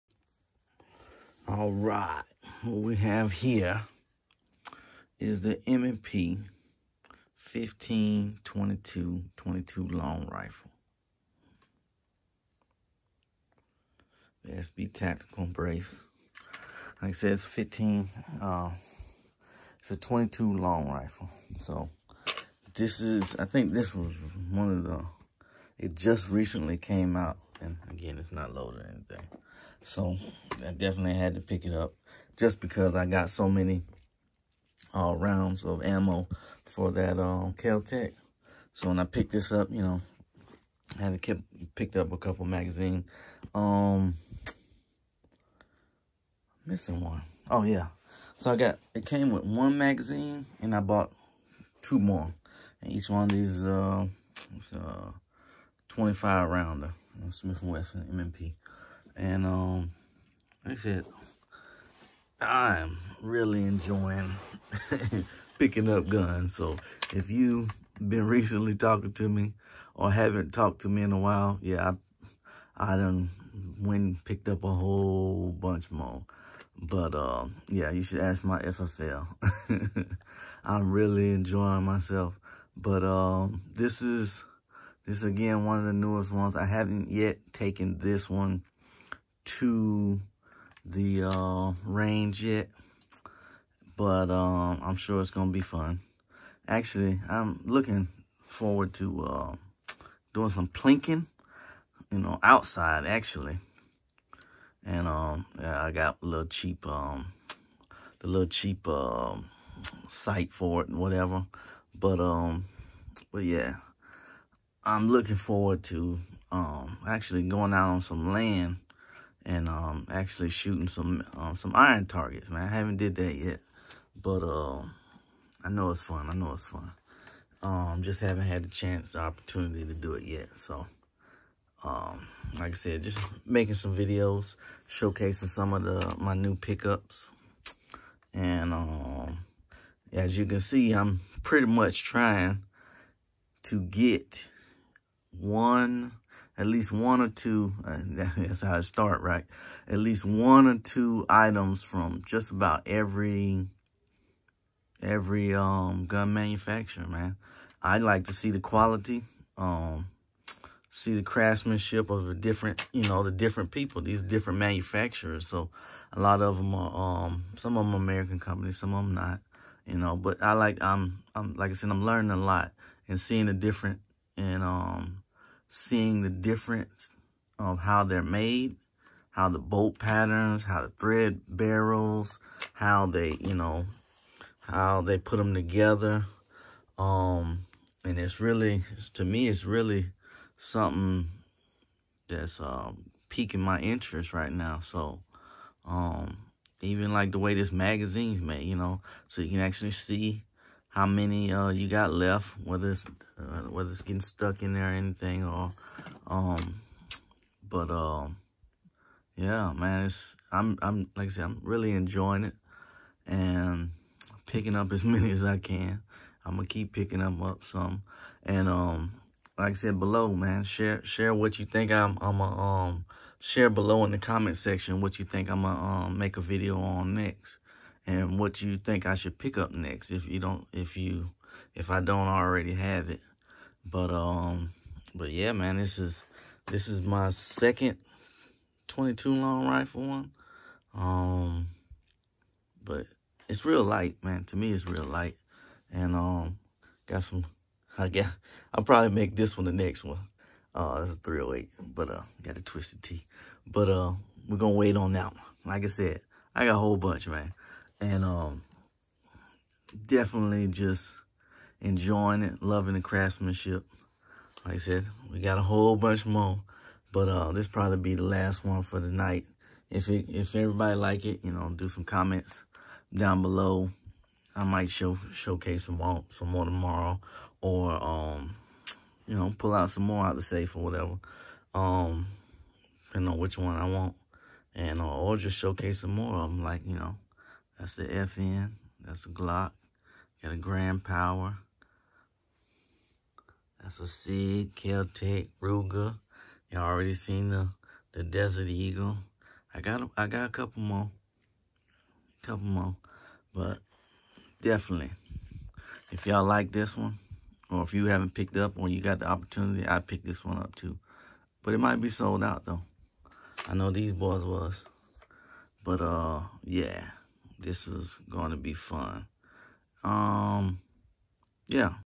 Smith & Wesson M&P 15-22 (Quick Unbox)
My second 22LR pickup this year. This one to help my wife and daughter get used to bigger guns. Bonus: .308 video may come next.